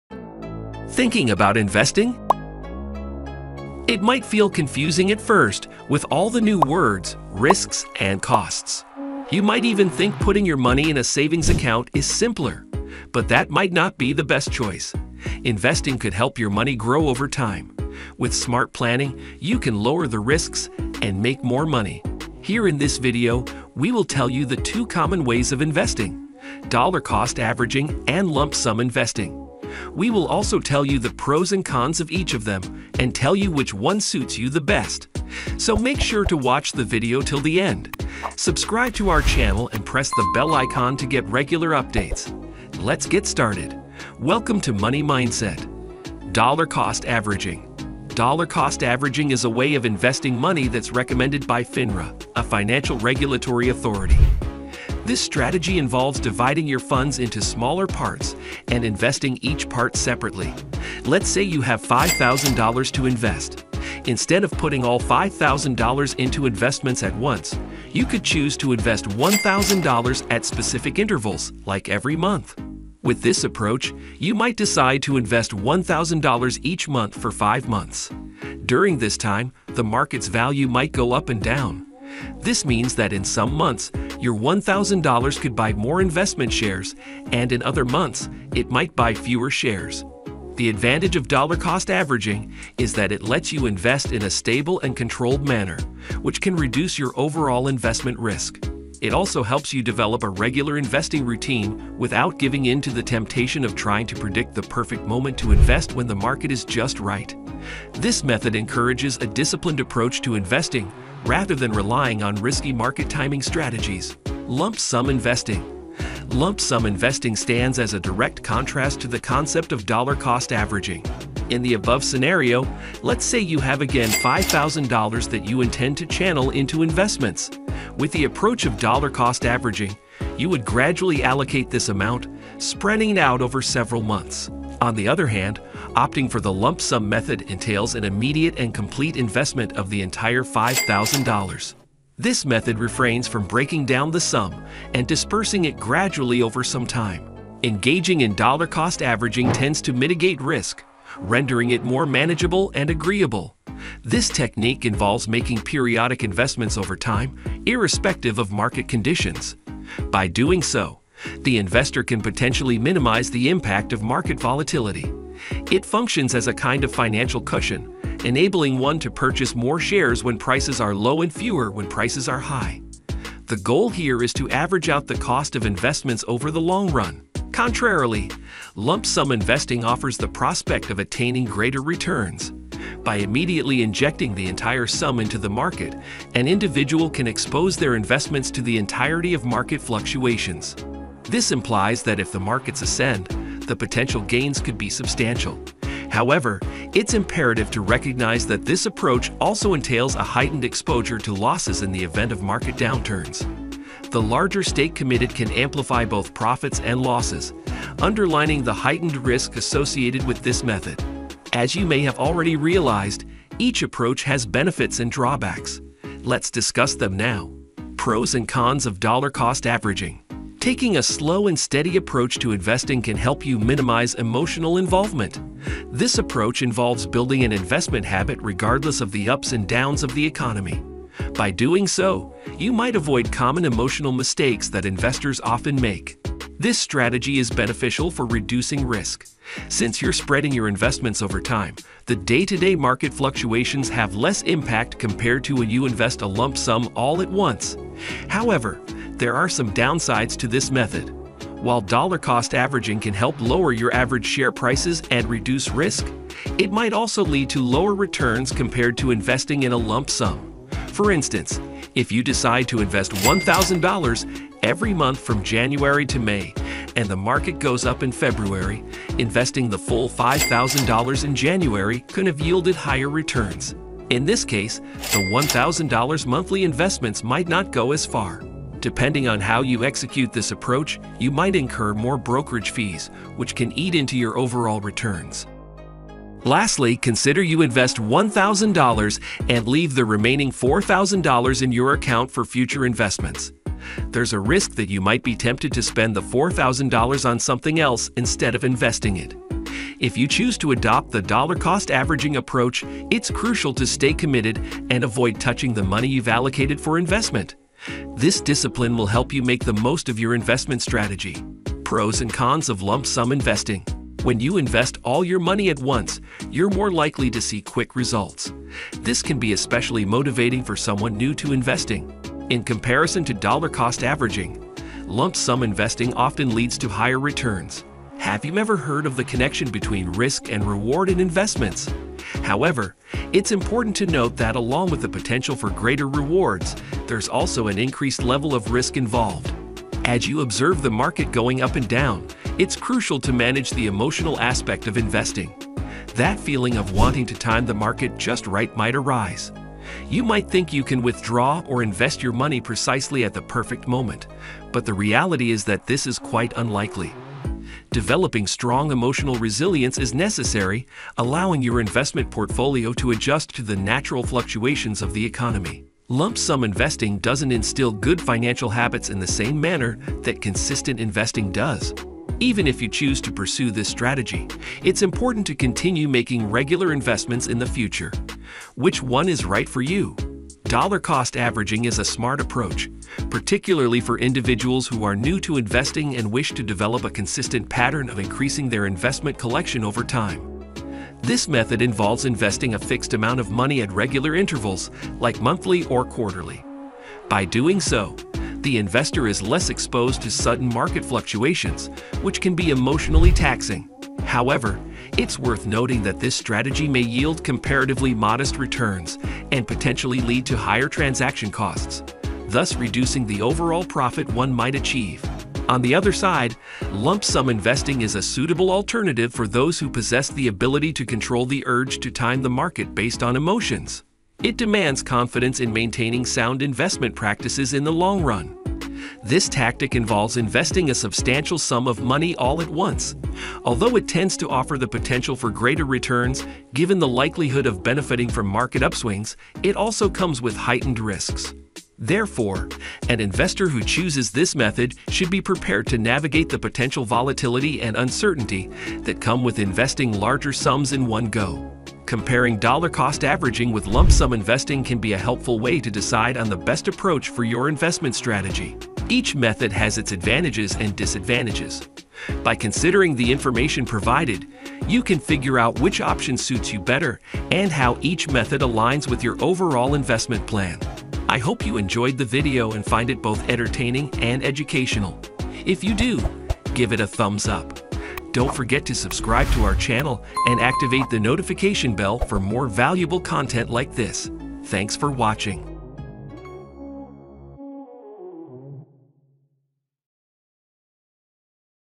Revealing secrets: The truth about Dollar cost averaging & lump sum investing
"In this video, we explore the age-old debate between dollar cost averaging and lump sum investing. We break down the pros and cons of each strategy, helping you make an informed decision on how to approach your investments. Whether you're a seasoned investor or just starting out, this video will provide valuable insights to help you navigate the world of investing. Watch now to learn more and make the best decision for your financial future!"